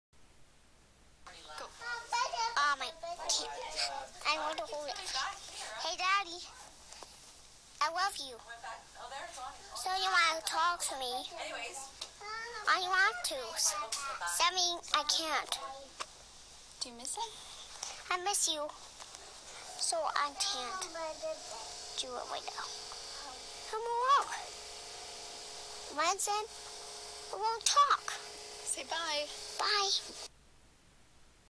Gabby misses Daddy
Im away this week for work and was sent this on behalf of my 3 year old daughter.

She was talking on an iphone while it was recording.  She thought it was a phone call and was wondering why i wasn't replying to her.